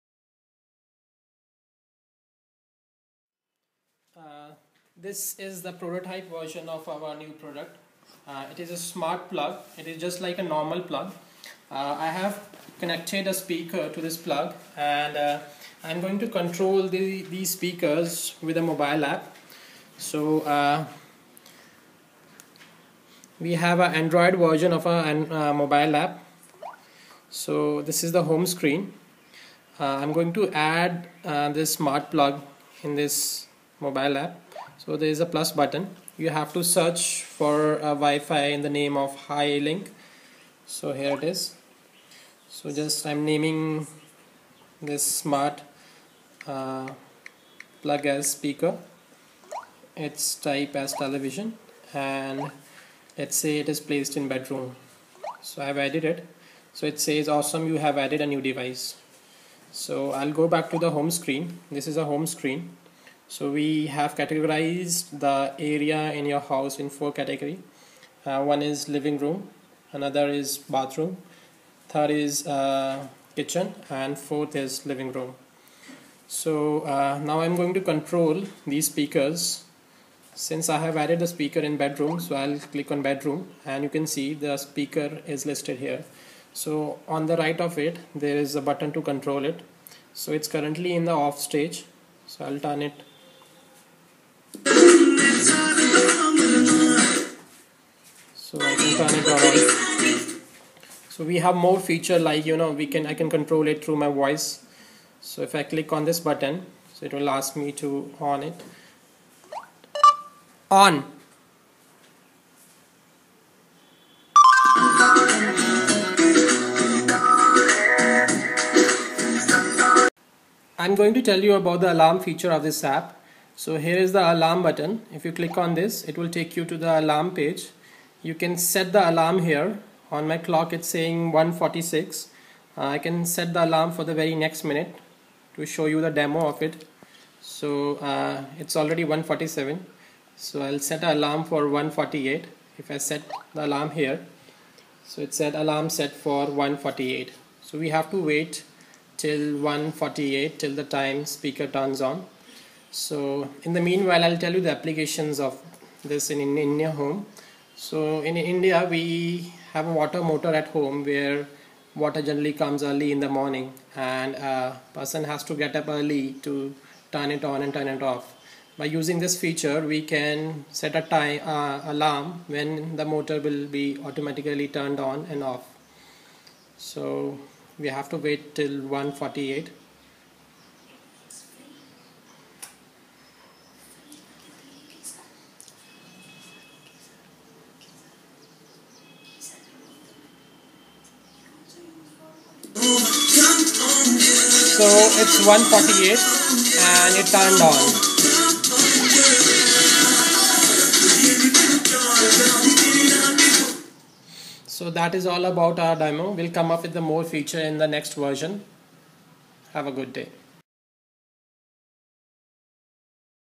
Smart Plug
Smart Plug is a 3 pin power plug controlled by a mobile app.
Features:
* Turn on/off devices remotely
* Schedule devices on/off by alarm feature
* Voice control
* Energy Monitoring
*Proximity sensor to turn on the light when you enter the room and turn off when you leave

Use cases:
*Turn water motor on/off at specific time
*Auto disconnect charger of the devices on fully charged
*Turn on geyser at specific time
*Turn on AC remotely from outside home during summers to cool down house before you arrive
* Turn on lights of home at night when you are on vacations
More features depends on the creativity of the users.